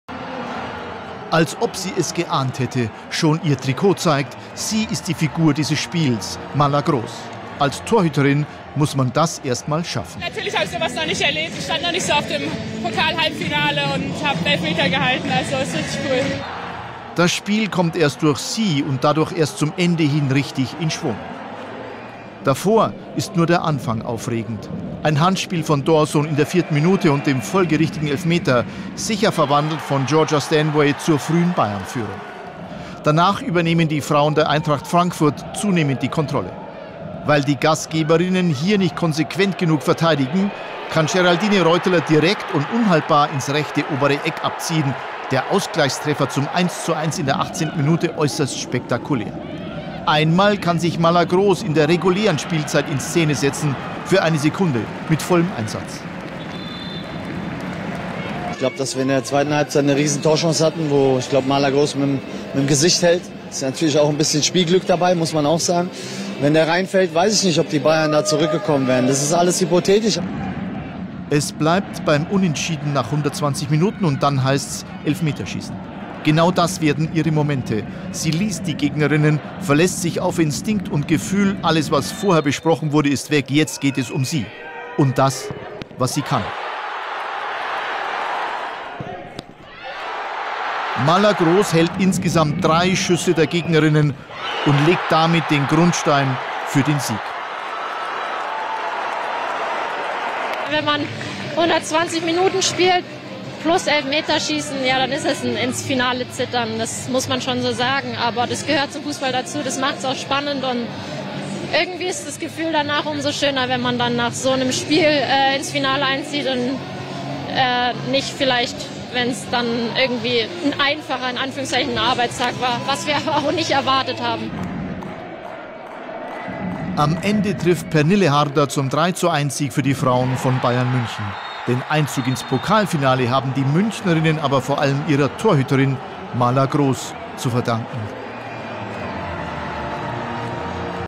FC Bayern München - Eintracht Frankfurt Highlights DFB-Pokal, Halbfinale I Sportschau Fußball
Die deutschen Meisterinnen vom FC Bayern München sind dem VfL Wolfsburg dank Torhüterin Maria Luisa Grohs ins Endspiel des DFB-Pokals gefolgt und haben das erste Traumfinale seit 2018 perfekt gemacht.

Die Münchnerinnen gewannen am Sonntag ihr Halbfinale gegen Eintracht Frankfurt mit 3:1 im Elfmeterschießen. Nach 120 und 90 Minuten hatte es 1:1 (1:1) gestanden. Die Wölfinnen waren am Vortag durch einen 9:0-Kantersieg gegen die SGS Essen zum zehnten Mal in Folge ins Finale eingezogen.

Bayerns Torfrau Maria Luisa Grohs parierte drei der vier Versuche der Frankfurterinnen. Den entscheidenden Elfmeter verwandelte Pernille Harder. Zuvor hatte Georgia Stanway die Bayern früh per Handelfmeter (4.) in Führung geschossen, Geraldine Reuteler (18.) aber schnell mit einem traumhaften Volleyschuss für die SGE ausgeglichen.

Die Vorzeichen für den zweiten Pokalsieg der Bayern-Frauen nach 2012 stehen gut, in der Liga gab es vor einer Woche einen 4:0-Erfolg im Prestigeduell gegen Wolfsburg. Im letzten Endspiel zwischen den beiden Teams setzte sich 2018 jedoch Seriensieger Wolfsburg mit 3:2 im Elfmeterschießen durch.

Die Partie begann vor den Augen des neuen Bundestrainers Christian Wück, DFB-Sportdirektorin Nia Künzer sowie Bayern-Präsident Herbert Hainer aufregend. Nach drei Minuten blockte Nationalspielerin Sara Doorsoun eine Flanke von Jovana Damnjanovic klar mit dem Arm. Schiedsrichterin Riem Hussein übersah das Vergehen, gab nach einem Hinweis der Linienrichterin aber den Strafstoß, den Stanway verwandelte.

Doch die Frankfurterinnen reagierten mutig und waren zweikampfstark, Reuteler gelang bei ihrem Ausgleichstreffer von der Strafraumkante ein Sonntagsschuss.

Die Bayern verpassten durch einen Kopfball von Schüller (23.) und einen Schuss von Damnjanovic (36.) die erneute Führung. Bis zum Elfmeterschießen aber gelang weder den Bayern noch der Eintracht der Siegtreffer.

Bayern München: Grohs - Gwinn, Viggosdottir, Sembrant, Naschenweng - Kerr, Stanway, Harder, Damnjanovic, Bühl - Schüller

Eintracht Frankfurt: Johannes - Wolter, Doorsoun, Kleinherne, Hanshaw - Reuteler, Gräwe, Dunst, Freigang - Anyomi, Prasnikar

Schiedsrichterin: Riem Hussein